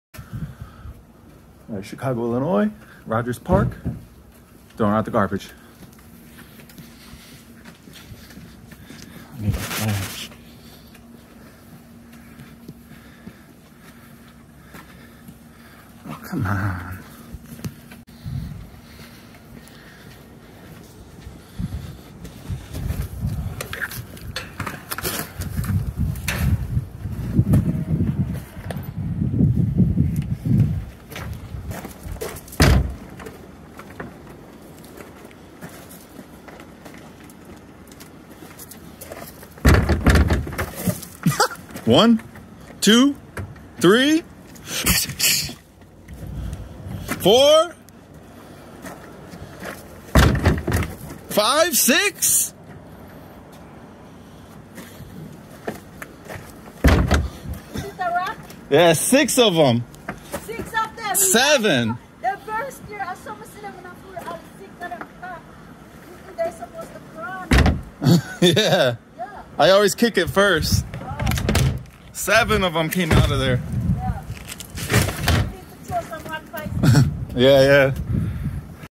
Rats in the Trash Chicago IL Rogers Park
Rats in the Trash

Every night when I get home from work I need to throw out the trash.  Just gotta give the trash can a little kick first. Make sure nothing jumps up at you. Which has happened before, and throwing trash on the rats too.